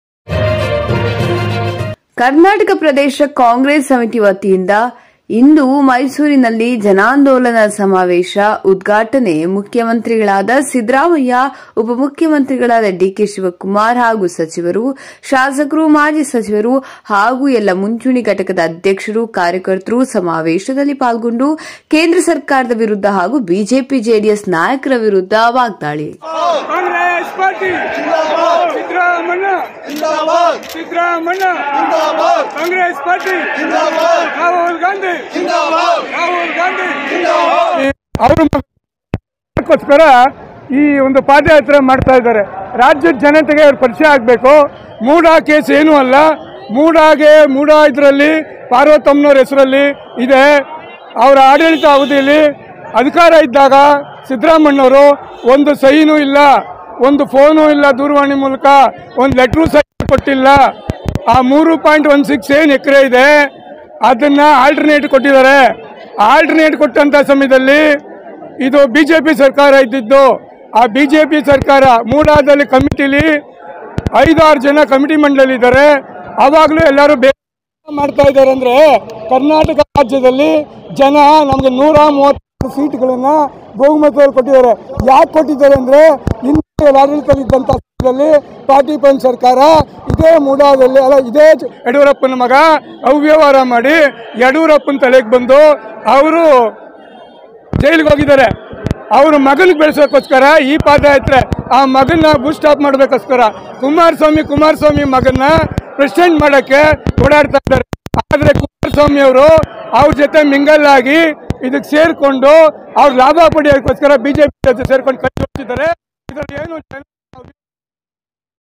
ಕರ್ನಾಟಕ ಪ್ರದೇಶ್ ಕಾಂಗ್ರೆಸ್ ಸಮಿತಿ ವತಿಯಿಂದ ಇಂದು ಮೈಸೂರಿನಲ್ಲಿ ಜನಾಂದೋಲನ ಸಮಾವೇಶ
ಕರ್ನಾಟಕ ಪ್ರದೇಶ್ ಕಾಂಗ್ರೆಸ್ ಸಮಿತಿ ವತಿಯಿಂದ ಇಂದು ಮೈಸೂರಿನಲ್ಲಿ ಜನಾಂದೋಲನ ಸಮಾವೇಶ   ಉದ್ಘಾಟನೆ ಮುಖ್ಯಮಂತ್ರಿಗಳಾದ ಸಿದ್ದರಾಮಯ್ಯ ಉಪ ಮುಖ್ಯಮಂತ್ರಿಗಳಾದ ಡಿಕೆ ಶಿವಕುಮಾರ್ ಹಾಗೂ ಸಚಿವರು . ಶಾಸಕರು. ಮಾಜಿ ಸಚಿವರು. ಹಾಗೂ ಎಲ್ಲಾ ಮುಂಚೂಣಿ ಘಟಕದ  ಅಧ್ಯಕ್ಷರುಗಳು ಕಾರ್ಯಕರ್ತರು ಸಮಾವೇಶದಲ್ಲಿ ಪಾಲ್ಗೊಂಡು  ಕೇಂದ್ರ ಸರ್ಕಾರದ ವಿರುದ್ಧ ಹಾಗೂ ಬಿಜೆಪಿ. ಜೆಡಿಎಸ್. ನಾಯಕರ ವಿರುದ್ಧ ವಾಗ್ದಾಳಿ